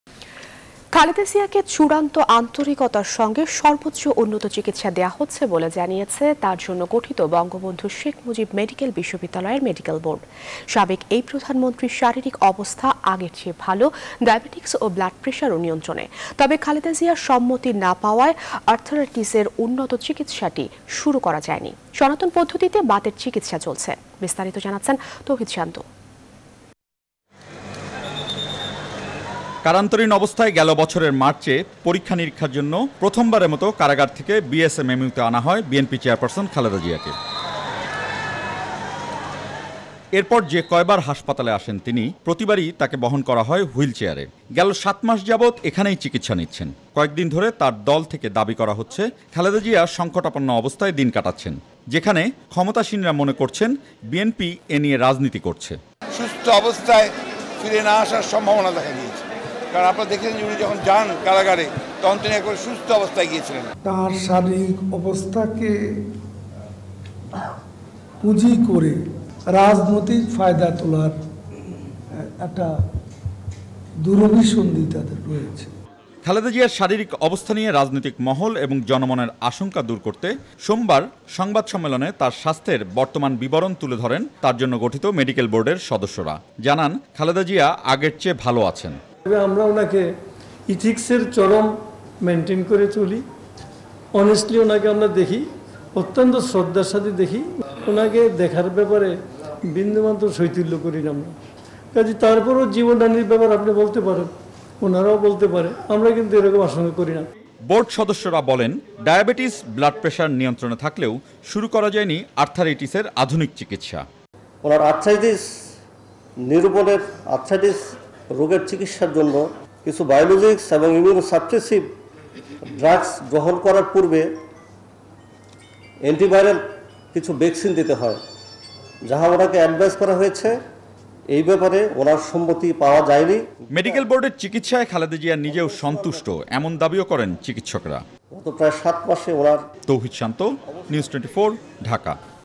সর্বোচ্চ উন্নত চিকিৎসা দেয়া হচ্ছে, জানালো মেডিকেল বোর্ড ।। মানতে নারাজ বিএনপি নেতারা 28Oct.19
খালেদা জিয়াকে চূড়ান্ত আন্তরিকতার সঙ্গে সর্বোচ্চ উন্নত চিকিৎসা দেয়া হচ্ছে বলে জানিয়েছে তার জন্য গঠিত বঙ্গবন্ধু শেখ মুজিব মেডিকেল বিশ্ববিদ্যালয়ের মেডিকেল বোর্ড। সাবেক এই প্রধানমন্ত্রীর শারিরীক অবস্থা আগের চেয়ে ভালো, ডায়াবেটিস ও ব্লাডপ্রেসারও নিয়ন্ত্রণে। তবে খালেদা জিয়ার সম্মতি না পাওয়ায় আর্থারাইটিসের উন্নত চিকিৎসাটি শুরু করা যায়নি, সনাতন পদ্ধতিতে বাতের চিকিৎসা চলছে।বিস্তারিত তৌহিদ শান্তর প্রতিবেদনে।

News24 Official Address:
NEWS24
371/A
Bashundhara Road
Block – D
Bashundhara Residential Area
Dhaka - 1229